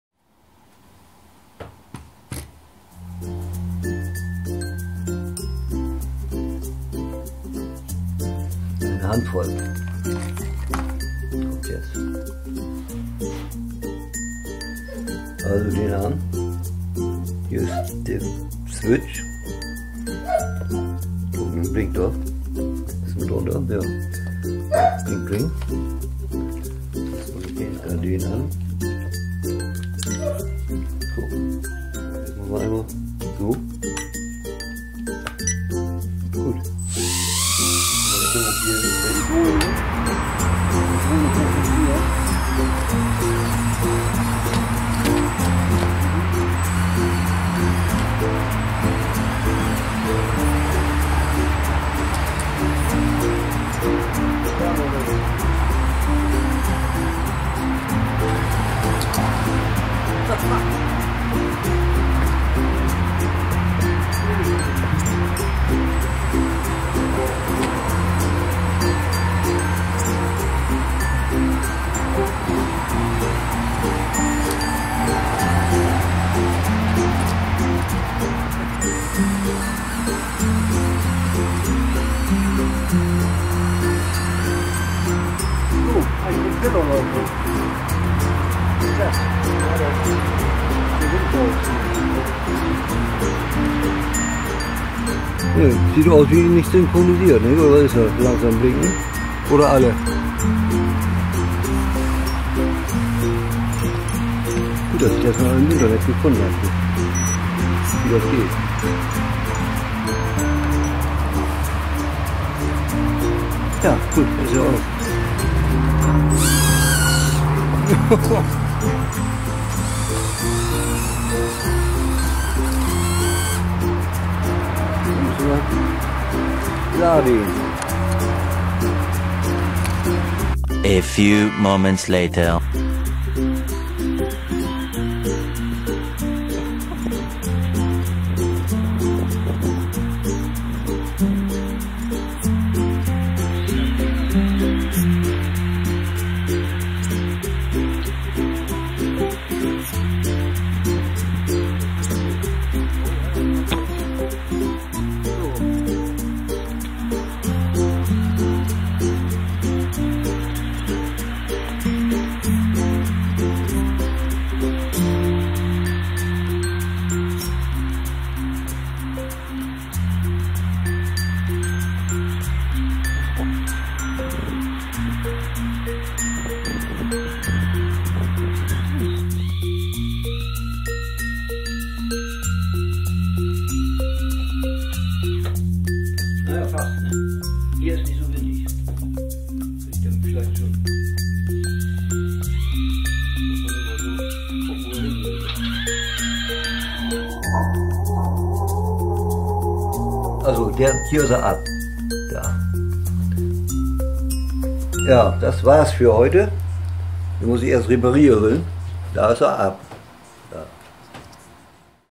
Pocket Drone 124 FQ777 - First Flight
Pocket Drone 124 - First Flight, too much wind!